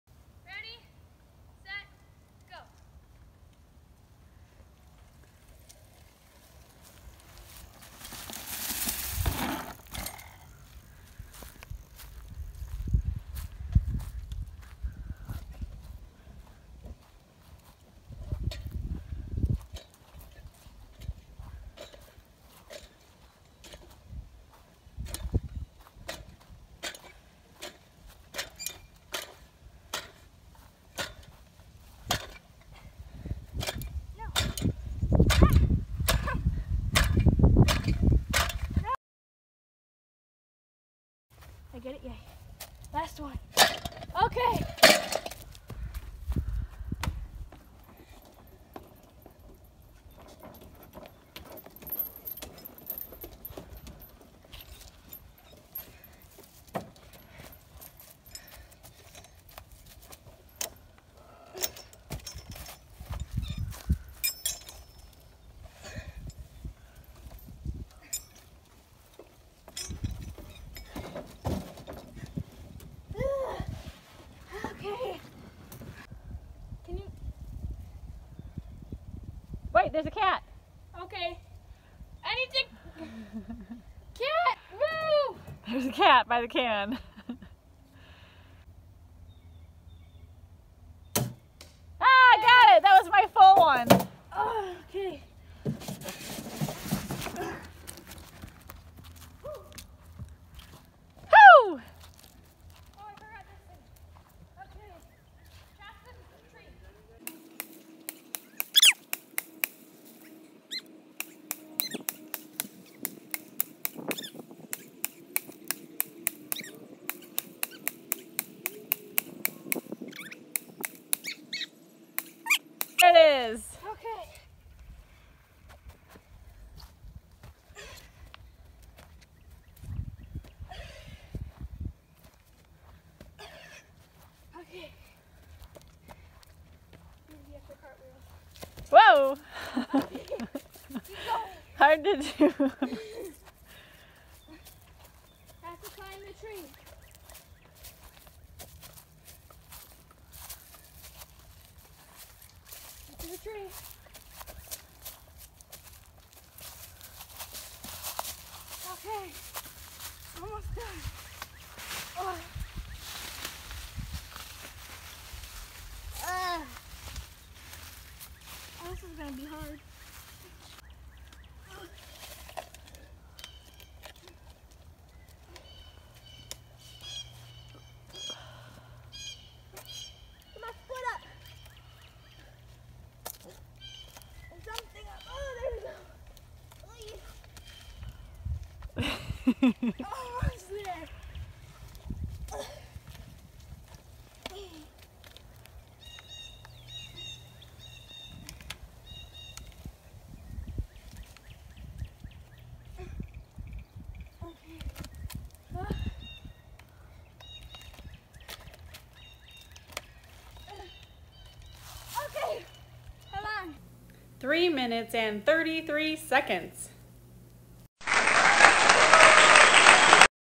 Doolin Moran's obstacle course
Hi!  Check out my obstacle course video I made for school!